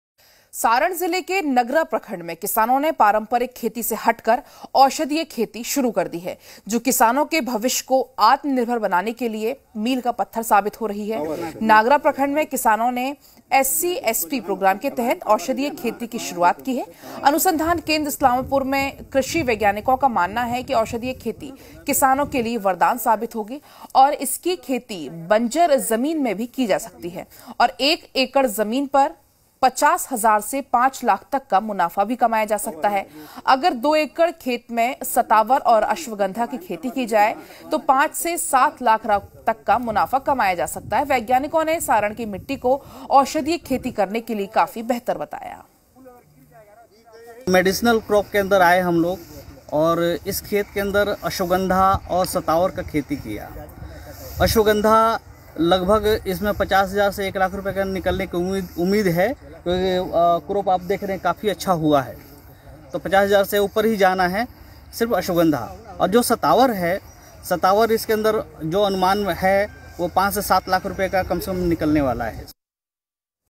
औषधीय खेती बढ़ते क्रम में छपरा बिहार द्वारा औषधीय खेती विकास संस्थान शाहजहाँपुर उत्तर प्रदेश
आज 21 जनवरी 2022 को ग्राम रसूलपुर अरवा नागरा प्रखंड जिला सारण बिहार में scsp प्रोग्राम के तहत औषधीय खेती का भर्मण पान व औषधीय सुगंधिये अनुसंधान केंद्र इस्लामपुर से वैज्ञानिकों द्वारा हुआ। जिसमें डॉक्टर एस एन दास, डॉक्टर अजीत पांडेय व डॉक्टर प्रभात कुमार मुख्य थे औषधीय खेती विकास संस्थान के बिहार झारखंड प्रभारी भी मौजूद थे। डॉक्टर एस एन दास ने कहा औषधीय खेती किसानों के लिए वरदान साबित हो रहा है उनुपजाउ जमीन पर औषधीय खेती करना संभव है और 1 एकड़ जमीन पर 50 हज़ार से 5 लाख तक आराम से कमाई किसान कर सकता है।
*औषधीय खेती विकास संस्थान* के तरफ से बिहार-झारखंड प्रभारी *श्री नंद लाल राम जी* ने किसानों को संबोधित किया और औषधीय खेती के फायदे बताये किस तरह से एक साथ 2 फसल किया जा सकता है वो खेत मे दिखाए
यहाँ 2 एकड़ खेत मे सतावर और अश्वगंधा की खेती किया गया है जिसमे अश्वगंधा अब हार्वेस्टिंग के लिए तैयार है अश्वगंधा का जड़ उच्च कोटि का पेंसलनुमा जड़ का उत्पादन हुआ है बिना कोई ब्रांच के।
अनुमान लगाया जगह है 1 एकड़ खेत से 1 लाख तक का उत्पादन हो जाएगा और 18 महीने बाद सतावर की हार्वेस्टिंग किया जाएगा जिससे किसान को लगभग 5 से 7 लाख तक कमाई होने की संभावना है। इस फसल का *Buyback* गारण्टी के साथ *औषधीय खेती विकास संस्थान* उपलब्ध करवाएगी।
सभी किसानों ने सहमति जताई है आने वाले समय मे इस खेती को करेंगे।
*डॉक्टर अजीत पांडेय और डॉक्टर प्रभात कुमार* ने किसानों को संबोधित किया सारण की मिट्टी औषधीय खेती करने के लिए अति उत्तम है सभी किसानों को इसकी खेती करना चाहिए जिससे बिहार से पलायन को रोका जा सके बिहार में राह कर ही युवा इस खेती से किसी भी नौकरी से ज्यादा कमा सकते है!
य
अधिक जानकारी के लिए कॉल कीजिये-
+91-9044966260
*औषधीय खेती विकास सन्स्थान*